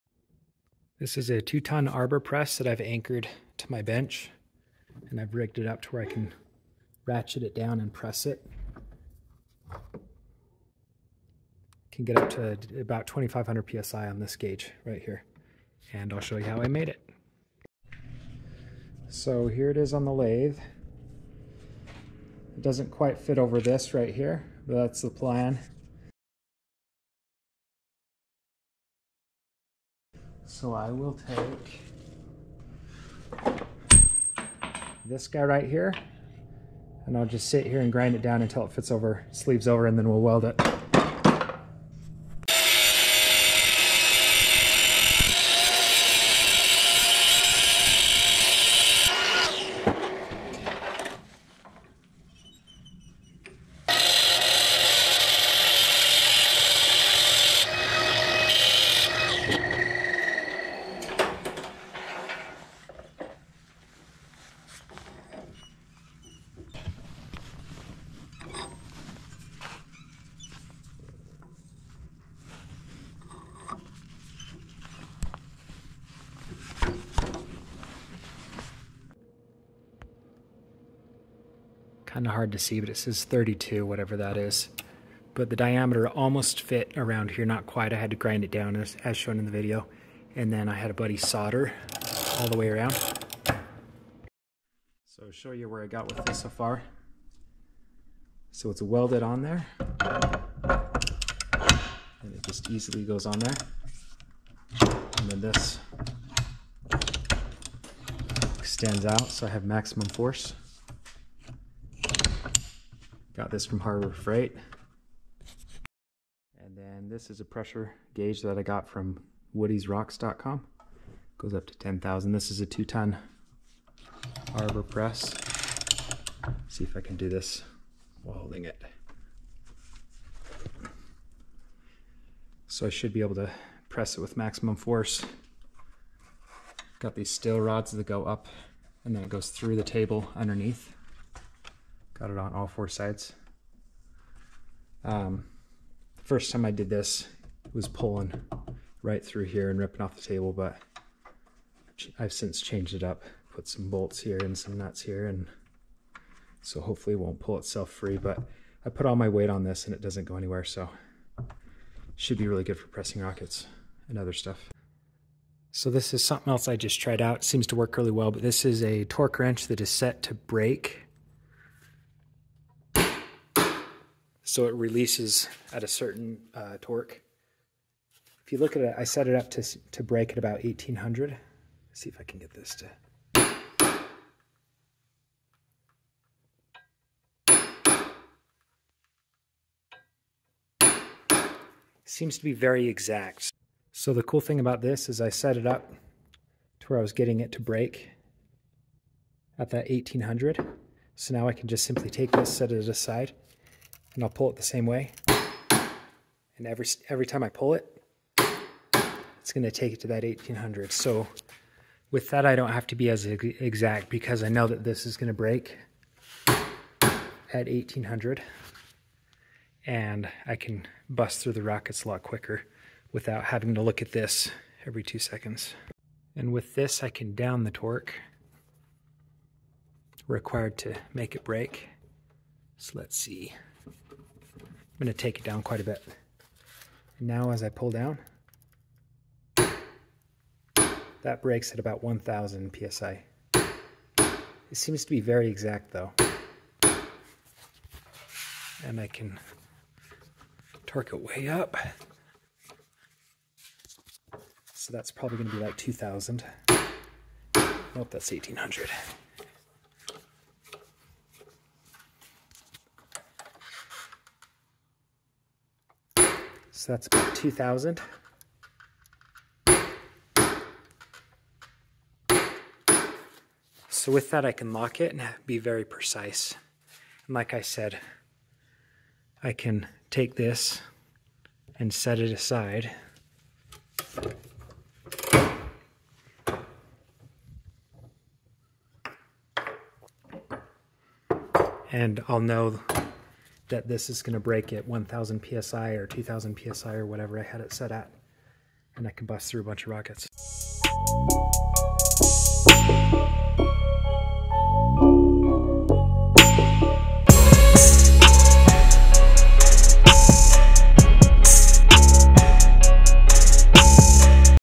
2 ton Arbor Press mod for fireworking. pressing rockets, stars, pyrotechnics
2 ton Arbor Press modded out to fit rocket tooling.   pyrotechnics, fireworking, rockets, black powder,